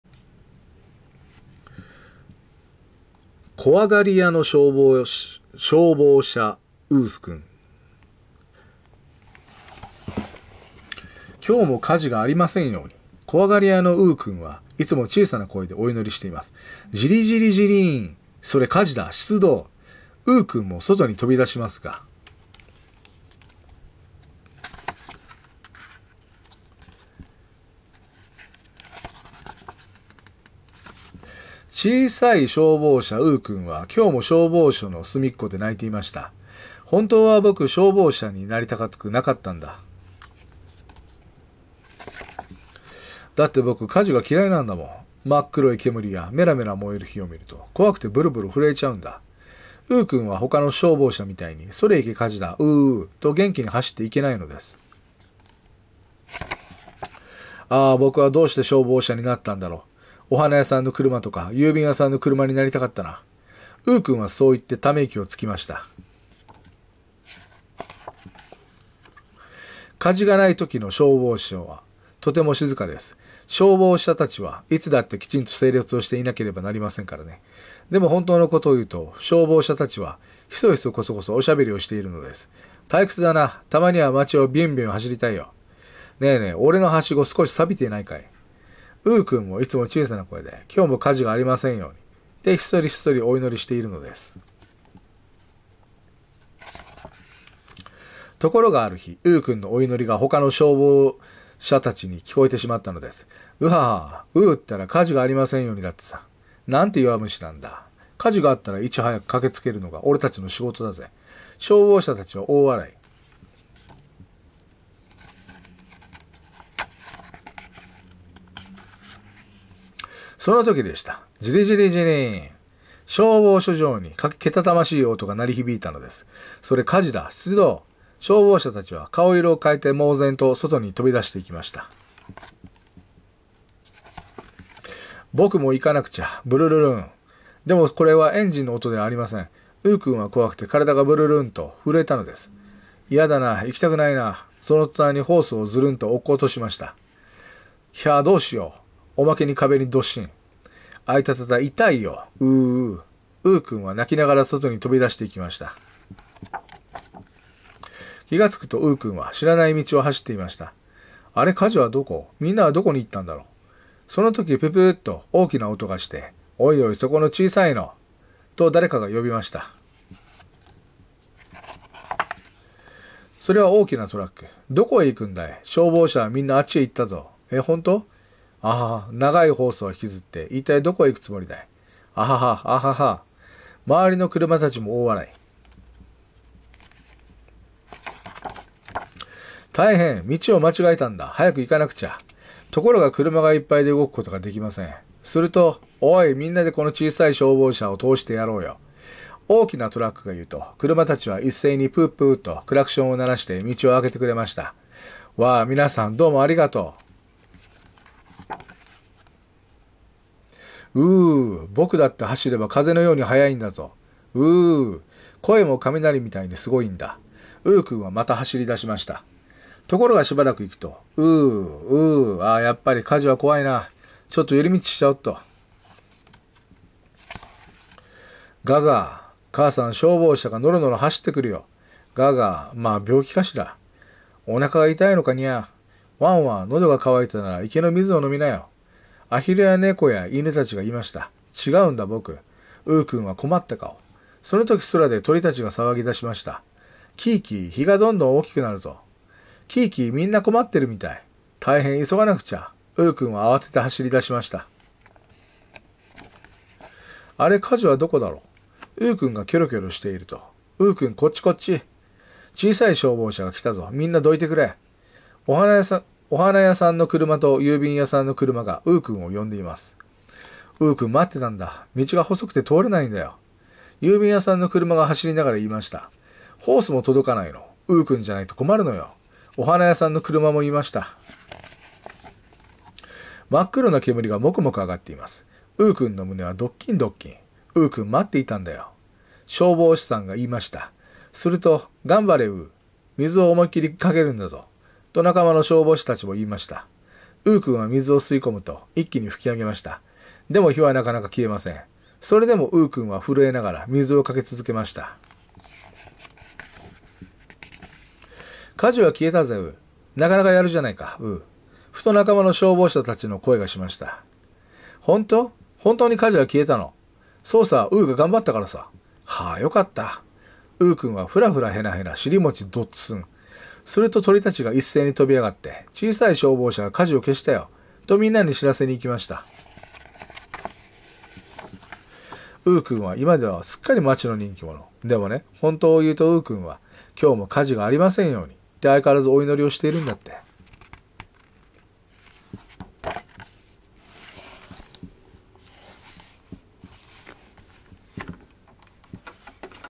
こわがりやのしょうぼうしゃううくん
「きょうも、かじが
ありませんように……」
こわがりやの　ううくんは
いつも　ちいさな　こえで、
おいのりしています。

ジリジリジリーン！
「それっ、かじだ、しゅつどう！」
ううくんも
そとに　とびだしますが……。

作　戸田和代
絵　にしかわおさむ

ちいさい　しょうぼうしゃ、ううくんは、
きょうも　しょうぼうしょの　すみっこで　ないていました。

「ほんとうは　ぼく、しょうぼうしゃに　なりたくなかったんだ……」

「だって　ぼく、かじが　きらいなんだもん。
まっくろい　けむりや、めらめら　もえる　ひを　みると、
こわくて　ぶるぶる　ふるえちゃうんだ」
ううくんは、ほかの　しょうぼうしゃみたいに、
『それいけ、かじだ！　ウーウーウー！』
と、げんきに　はしっていけないのです。

「あーあ、ぼくは　どうして　しょうぼうしゃに　なったんだろう。
おはなやさんの　くるまとか、ゆうびんやさんの
くるまに　なりたかったな……」
ううくんは、そう　いって　ためいきを　つきました。

かじが　ないときの　しょうぼうしょは、とっても　しずかです。
しょうぼうしゃたちは、いつだって　きちんと　せいれつを
していなければ　なりませんからね。
でも、ほんとうの　ことを　いうと、しょうぼうしゃたちは
ひそひそ　こそこそ　おしゃべりを　しているのです。
「たいくつだな。たまには　まちを　びゅんびゅん　はしりたいよ」
「ねえねえ、おれの　はしご、すこし　さびてないかい？」

ううくんも、いつも　ちいさな　こえで、
「きょうも、かじが　ありませんように……」
って、ひっそり　こっそり
おいのりしているのです。

ところが、あるひ、ううくんの　おいのりが
ほかの　しょうぼうしゃたちに　きこえてしまったのです。
「うははは！　ううったら、かじが　ありませんように、だってさ」
「なーんて　よわむしなんだ！」
「かじが　あったら、いちはやく　かけつけるのが、おれたちの　しごとだぜ」
しょうぼうしゃたちは　おおわらい。

そのときでした。
しょうぼうしょじょうに、けたたましい　おとが　なりひびいたのです。
しょうぼうしゃたちは、かおいろを　かえて、
もうぜんと　そとに　とびだしていきました。

「ぼ、ぼくも　いかなくちゃ。ぶるるるん」
でも、これは　エンジンの　おとでは　ありません。
ううくんは、こわくて　からだが　ぶるるるんと　ふるえたのです。
「いやだなあ、いきたくないなあ……」
そのとたんに　ホースを　ずるんと　おっことしました。

「ひゃあ、どうしよう！」
おまけに　かべに
どっしん。

「あいたたた、いたいよう、
ゥーゥーゥー……」
ううくんは、
なきながら　そとに
とびだしていきました。

きがつくと、ううくんは　しらない　みちを　はしっていました。
「あれ、かじは　どこ？　みんなは　どこに　いったんだろう？」
そのとき、ぷっぷーと　おおきな　おとが　して、
「おいおい、そこの　ちいさいの」
と、だれかが　よびました。

それは　おおきな　トラック。
「どこへ　いくんだい。しょうぼうしゃは、みんな　あっちへ　いったぞ」
「え、ほんとう！」
「あはは！　ながい　ホースを　ひきずって、いったい　どこへ　いくつもりだい？」
「あっはっは！」「あっはっは！」
まわりの　くるまたちも　おおわらい。

「たいへん、みちを　まちがえたんだ！はやく　いかなくちゃ！」
ところが、くるまが　いっぱいで　うごくことが　できません。
すると、
「おーい、みんなで　この　ちいさい　しょうぼうしゃを　とおしてやろうよ」
おおきな　トラックが　いうと、くるまたちは、いっせいに　ぷっぷーと
クラクションを　ならして、みちを　あけてくれました。
「わあ、みなさん、どうも　ありがとう！」

「ウーウーウー！」　ぼくだって、
はしれば　かぜのように　はやいんだぞ。
ウーウーウー！
こえも　かみなりみたいに　すごいんだ」
ううくんは、また　はしりだしました。

ところが、しばらく　いくと、
「ウーウーウー、ウーウーウー。
ああ、やっぱり　かじは　こわいなあ……。
ちょっと　よりみちしちゃおっと」

「ガーガー、かあさん、しょうぼうしゃが　のろのろ　はしってくるよ」
「ガーガー、まあびょうきかしら！」
「おなかが　いたいのかニャア？」
「ワンワン、のどが　かわいたなら、いけの　みずを　のみなよ」
あひるや　ねこや　いぬたちがいいました。
「違うんだ、ぼくは……」
ううくんは　こまった　かお。

そのとき、そらで　とりたちが　さわぎだしました。
「キイキイ、ひが　どんどん　おおきくなるぞ！
キイキイ、みんな　こまってるみたい」
「たいへん、いそがなくちゃ！」
ううくんは、あわてて　はしりだしました。

「あれ、かじは　どこだろう？」
ううくんが　きょろきょろしていると、
「ううくん、こっち　こっち！」
「ちいさい　しょうぼうしゃが　きたぞ。みんな　どいてくれ！」
おはなやさんの　くるまと、ゆうびんやさんの　くるまが、
ううくんを　よんでいます。

「ううくん、まってたんだ。みちが　ほそくて、とおれないんだよ」
ゆうびんやさんの　くるまが、はしりながら　いいました。
「ホースも　とどかないの。ううくんじゃないと　こまるのよ」
おはなやさんの　くるまも　いいました。

まっくろな　けむりが　もくもく　あがっています。
ううくんの　むねは　どっきん　どっきん。
「ううくん、まっていたんだよ！」
しょうぼうしさんが　いいました。すると、
「がんばれ　うう！」
「みずを　おもいっきり　かけるんだぞ！」
と、なかまの　しょうぼうしゃたちも　いいました。
ううくんは、みずを　すいこむと、いっきに　ふきあげました。
でも、ひは　なかなか　きえません。それでも、
ううくんは、ふるえながら　みずを　かけつづけました。

「かじは　きえたぜ、うう！」
「なかなか　やるじゃないか、うう！」
ふと、なかまの　しょうぼうしゃたちの　こえが　しました。

「ほんとう？　ほんとうに　かじは　きえたの？」
「そうさ、ううが　がんばったからさ」
「はあ～、よかった～！」
ううくんは、ふらふら　へなへな、しりもち　どっすん。
すると、とりたちが　いっせいに　とびあがって、
「ちいさい　しょうぼうしゃが、かじを　けしたよ！」
と、みんなに　しらせにいきました。

ううくんは、いまでは　すっかり　まちの　にんきもの。
でもね、ほんとうを　いうと、ううくんは
って、あいかわらず　おいのりを　しているんだって。

２０１３ねん４がつ　はっこう
はっこうしょ　ポプラしゃ
こどもえほんランド５
ていか：ほんたい１，３００えん　ぜいべつ